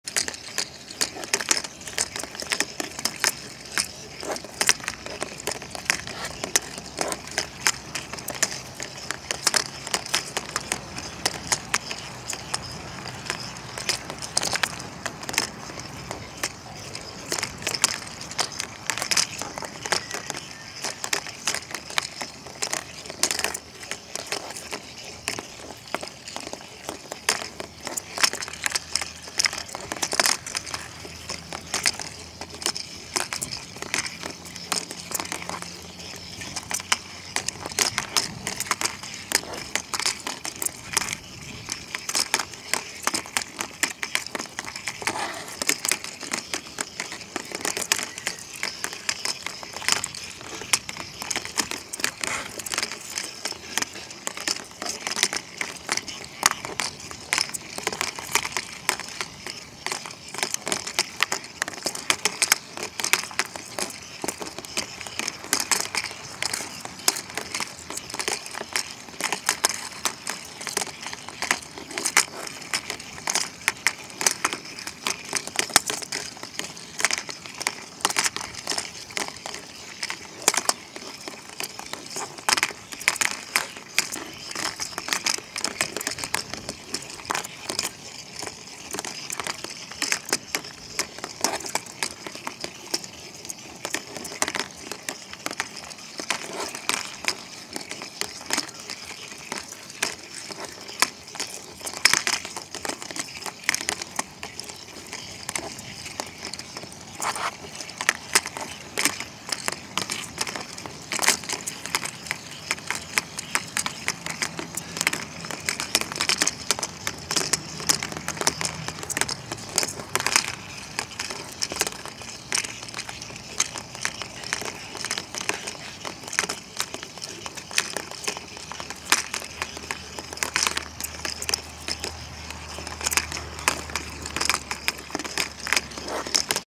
Weasels show (belettes)
Deux belettes et leurs baballes
Two weasels playing with balls